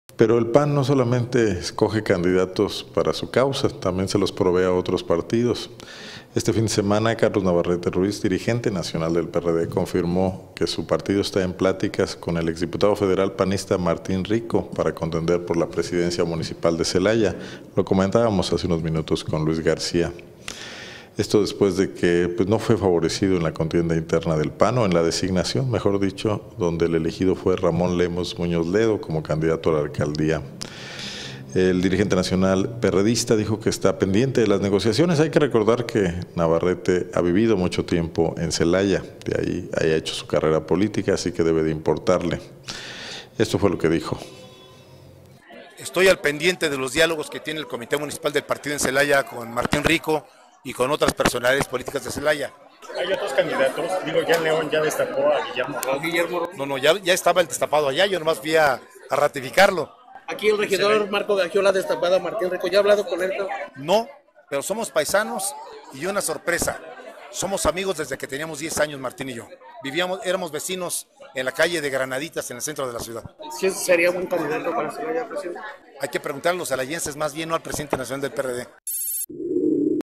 PRD Dialoga con Martín Rico
Celaya, Gto. El líder nacional del PRD, Carlos Navarrete Ruiz, confirmó que el partido que dirige está en pláticas con el exdiputado federal Martín Rico para contender por la presidencia municipal de Celaya.